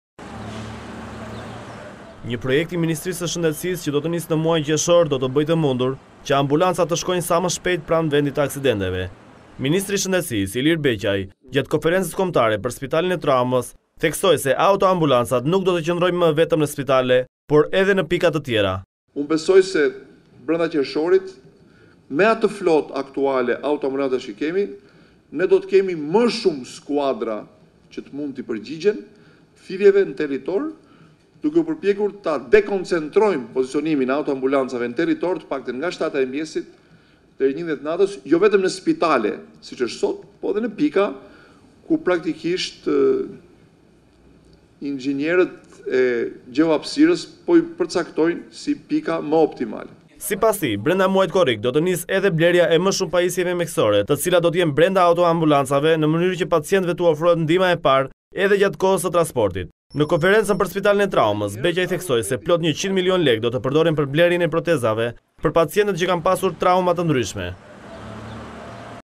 Report TV - Beqaj: 100 milion lekë për blerjen e protezave të Spitalit të Traumës
Ministri i Shëndetësisë, Ilir Beqaj ka thënë sot se, 100 milion lekë janë vënë në dispozicion të blerjes së protezave në Spitalin e Traumës.

Kjo ka qenë deklarata e bërë nga Ministri i Shëndetësisë Ilir Beqaj sot gjatë Konferencës së 2-të Kombëtare të Traumës.

“Nëse spitali nuk ka sot proteza, kjo nuk është as faji i mjekëve dhe as përgjegjësi e mjekëve, por të paktën, me buxhetin e këtij viti, meqenëse këtu u fol se në fund të fundit duhen edhe para për të bërë më shumë për traumën, janë 100 milion lekë të reja në dispozicion, vetëm për të blerë proteza. Sigurisht, me të drejtë duhet të blihen sa më shpejt, por bashkë me përfaqësuesit e QSUT-së dhe Spitalit të Traumës kemi kaluar një periudhë të parë dhe na është dashur ta bëjmë sa më mirë dhe në mënyrë sa më të qëndrueshme që pastaj, nga ky vit e në vazhdim, të kthehet në një procedurë që bëhet në mënyrë shumë më rutinore,” ka pohuar gjatë fjalës së tij kreu i shëndetësisë.

Por, një tjetër projekt që do të nisë në qershor të këtij viti ka të bëjë me shpërndarjen e autoambulancave në territorin e vendit, në mënyrë që ato të shkojnë sa më shpejt pranë vendeve të aksidenteve, ndërsa aktualish, është shtuar edhe numri i mjekëve dhe infermierëve që do të punojnë në këto skuadra.

“Unë besoj se, brenda qershorit, me atë flotë aktuale autoambulancash që kemi, ne do të kemi më shumë skuadra që të mund t’i përgjigjen thirrjeve në territor, duke u përpjekur ta dekoncentrojmë pozicionimin e autoambulancave në territor të paktën nga ora 7:00 e mëngjesit deri në 11:00 të natës, jo vetëm në spitale siç është sot, por edhe në pika ku, praktikisht, inxhinierët e gjeo-hapësirës po përcaktojnë si pika më optimale,” theksoi Beqaj.

Sipas tij, brenda muajit korrik do të nisë edhe blerja e më shumë pajisjeve mjekësore të cilat do të jenë brenda autoambulancave në mënyrë që pacientëve tu ofrohet ndihma e parë edhe gjatë kohës së transportit.

“Padyshim nuk mjafton që ajo të jetë vetëm një mjek motorik që shkon në kohë. Duhet të jetë e pajisur me pajisjet e nevojshme mjekësore për urgjencat dhe traumat. Brenda korrikut me një pjesë të buxhetit të shërbimit kombëtar të urgjencës ne do të pajisim disa autoambulanca me shumë pajisje tepër më të mira brenda” pohuar Beqaj, duke theksuar se gjatë këtyre viteve është ulur numri i traumave në rang vendi.